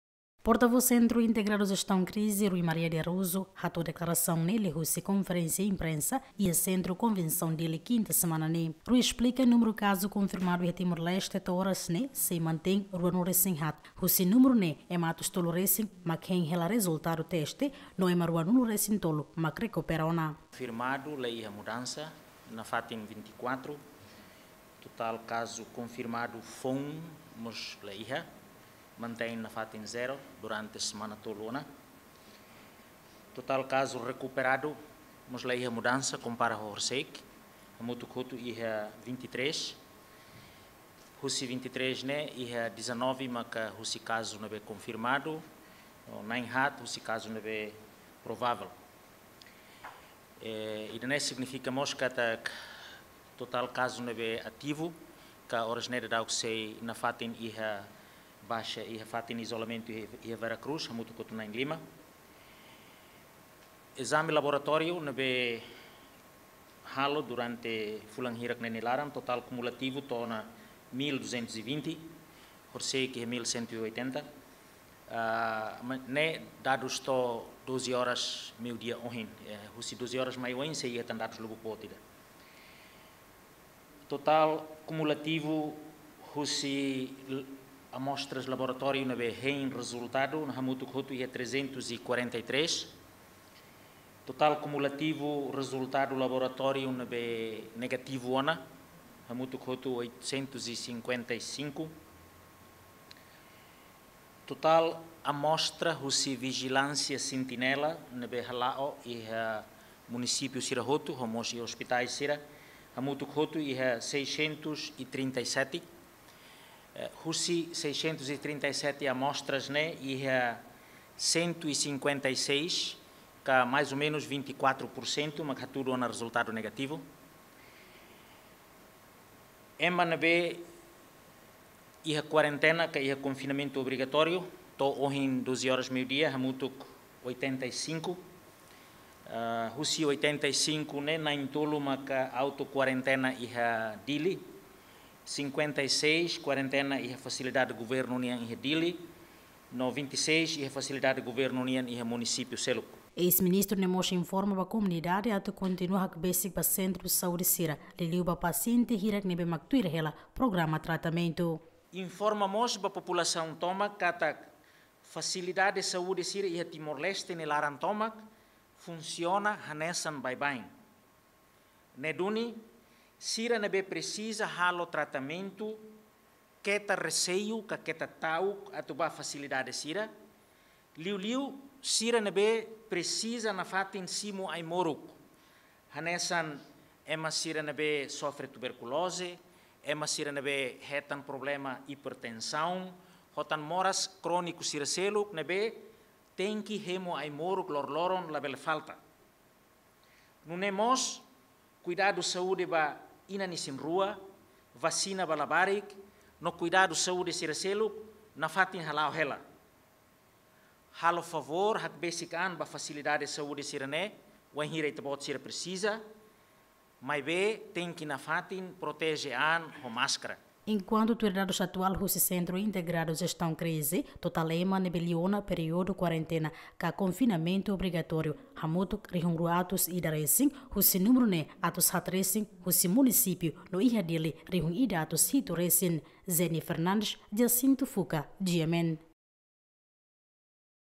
Rui Maria de Araujo: Ema atus tolu resin hein hela Rezultadu teste iha laboratóriu
Portavoz SIJK Rui Maria de Araujo: Ema atus tolu resin hein hela Rezultadu teste iha laboratóriu